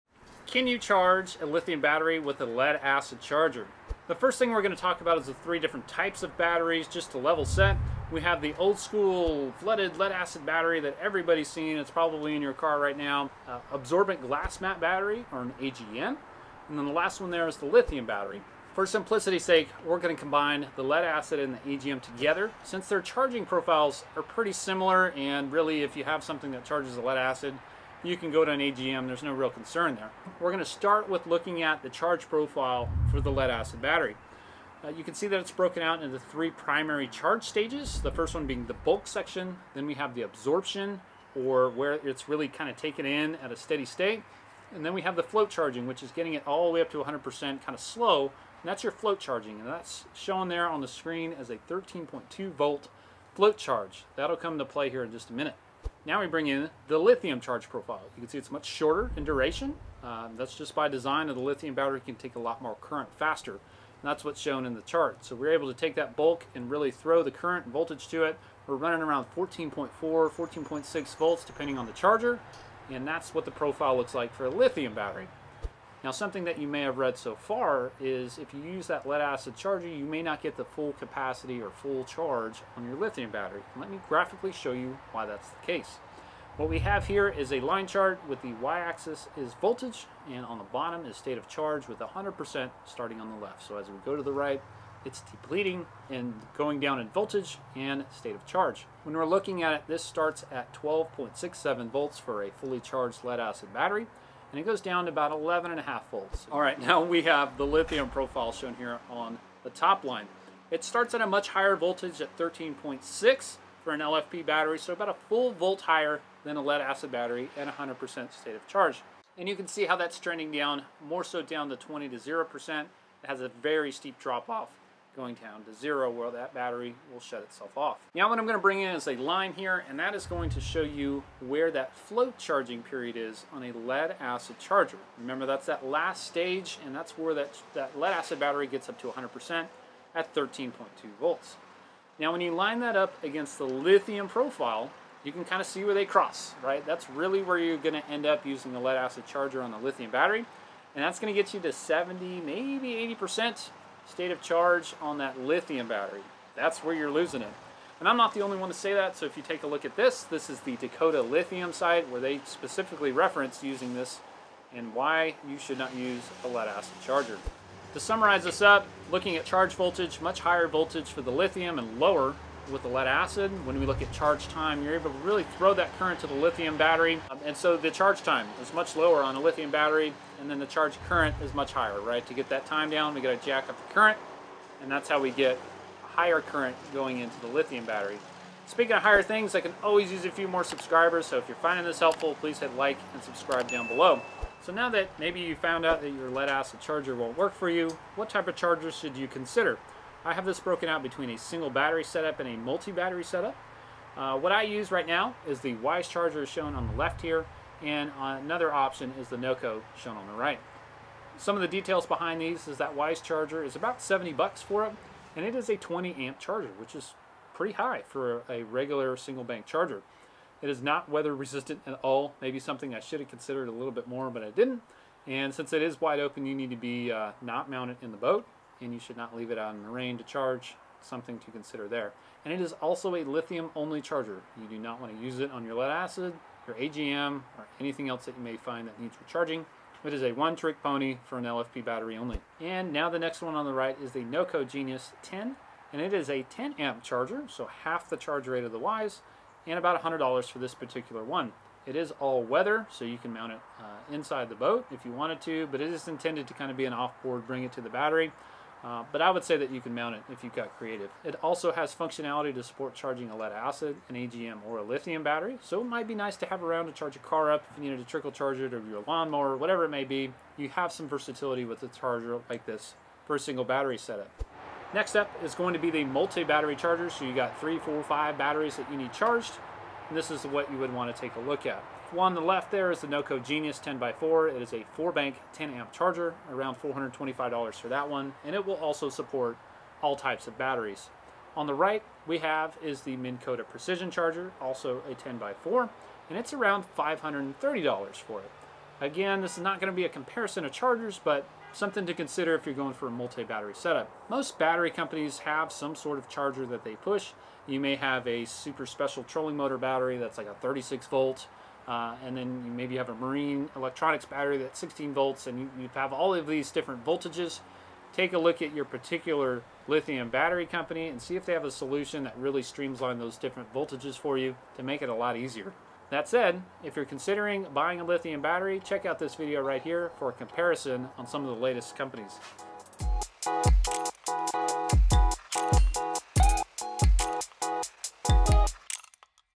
Can You Charge a LITHIUM Battery With a LEAD ACID Charger? (Should You?)
Lithium batteries are all the rage right now and they are EXPENSIVE! Most boats have existing Lead acid chargers and it would be nice to be able to reuse them, right? Watch the video to find out what you need to know to ensure those fancy Lithium, LFP, batteries perform for you the best they can. This applies to ANY LFP, LiFePo4 battery (Ionic, Weize, Battle-Born, Impulse, Dr. Prepare, Powerhouse, Dakota, Amped Outdoors, etc.)

Chargers Mentioned - Single Battery
► Weize 20a
► LiTime 10a
► Noco Genius 10

Chargers Mentioned - Multi-Battery
► Noco Genius 10x4
► Minn Kota 15x4 (10x4 mentioned)
► FirstPower Pro 10x3

► The Lithium Battery Referenced - Weize 100Ah, 12v

goodman_fishing is a participant in the Amazon Services LLC Associates Program,